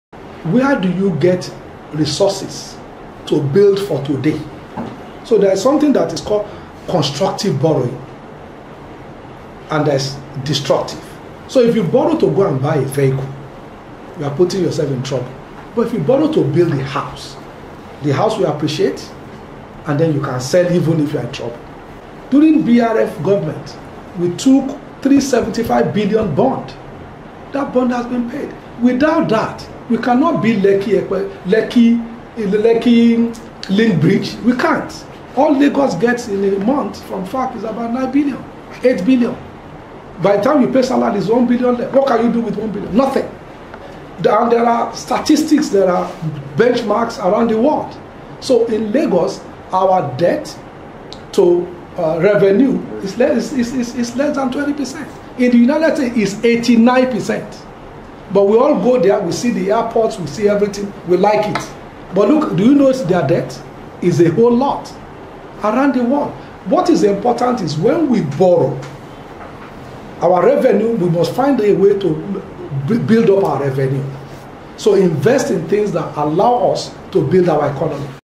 We Borrow Constructively in Lagos - Deputy Governor
Deputy Governor of Lagos State, Dr. Obafemi Hamzat speaks to the News Agency of Nigeria on the state's policy to borrow funds.

Filmed & Edited by Iyiola Pedro